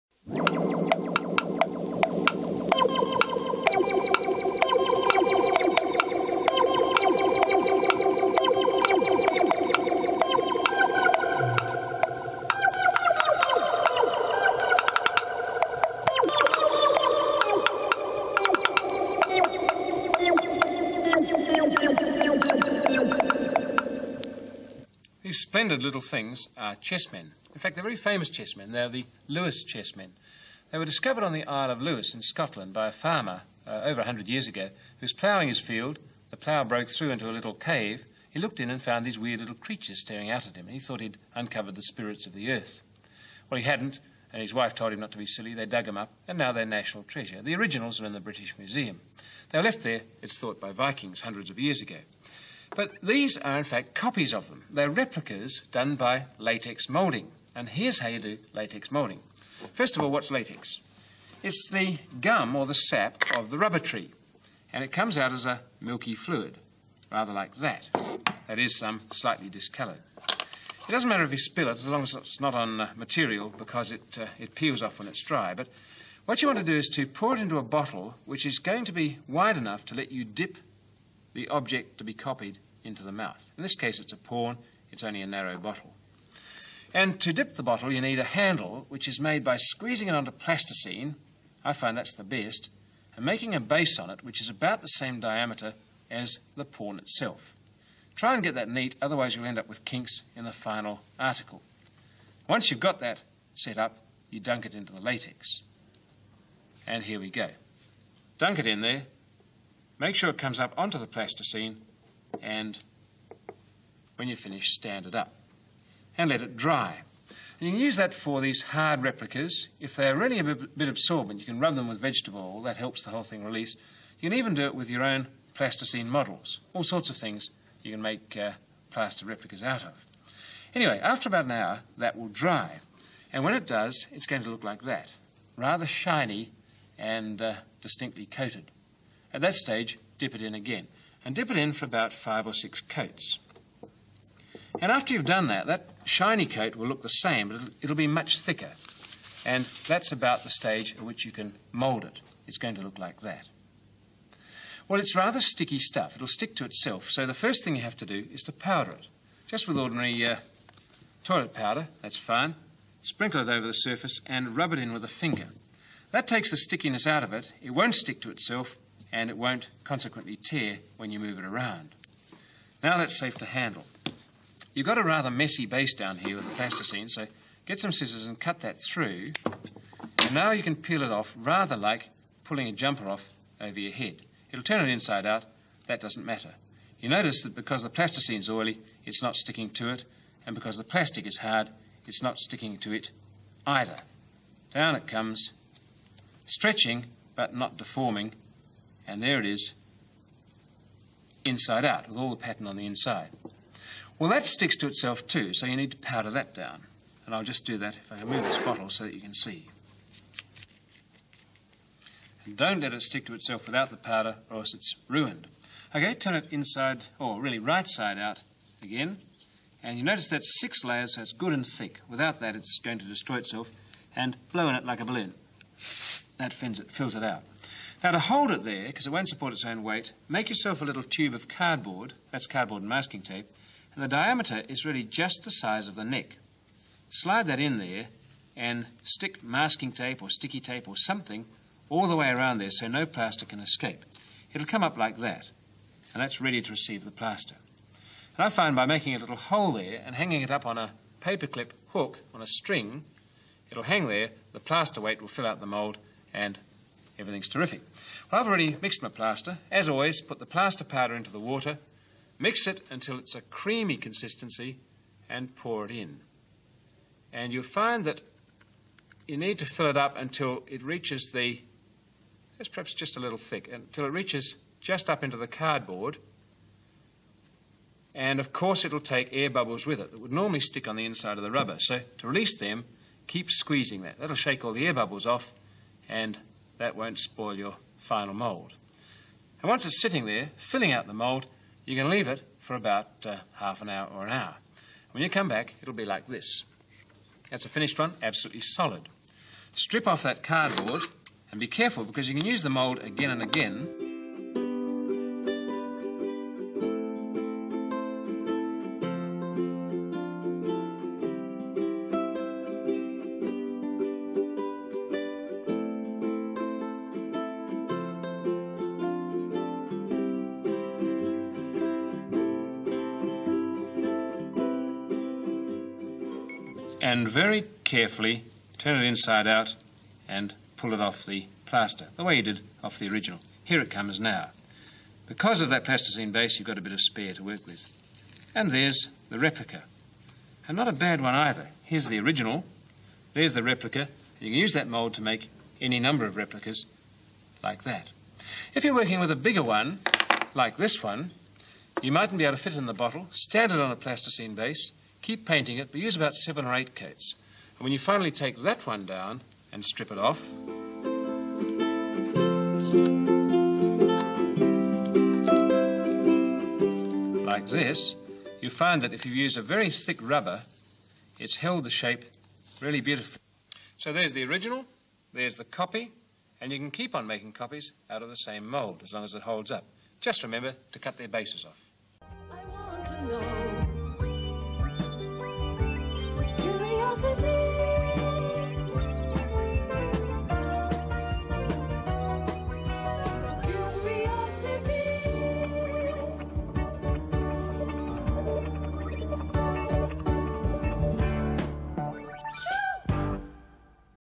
Cast your models with latex molds
Complicated models can be hard to reproduce but, as Rob shows, not with latex molds. Latex is pretty easy to obtain and using these simple tricks you can duplicate even complicated objects very easily.